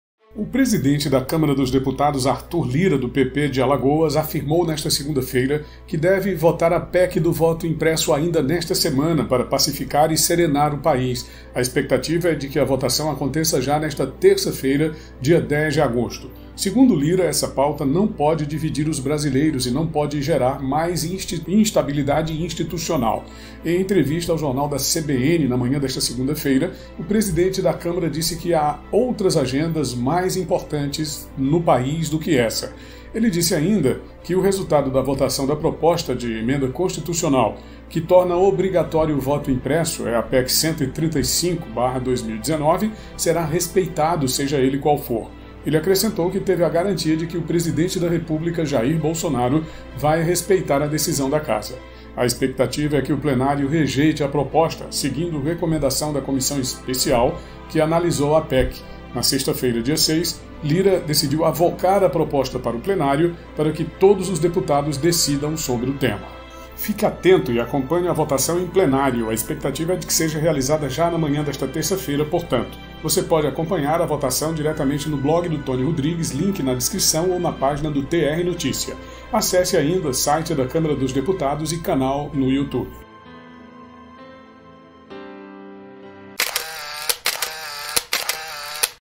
URGENTE PEC do voto impresso votação na Câmara acompanhe
A PEC do voto impresso deve ser votada a partir desta terça-feira no plenário da Câmara dos Deputados em Brasília. O presidente Arthur Lira disse que tem a garantia do presidente da República, Jair Bolsonaro, de que a votação do plenário será respeitada. Ele disse ainda que esta é a única forma de pacificar o país. Acompanhe a votação através do blog do Toni Rodrigues no Portal TV Piauí ou na página do TRNotícia (links na descrição).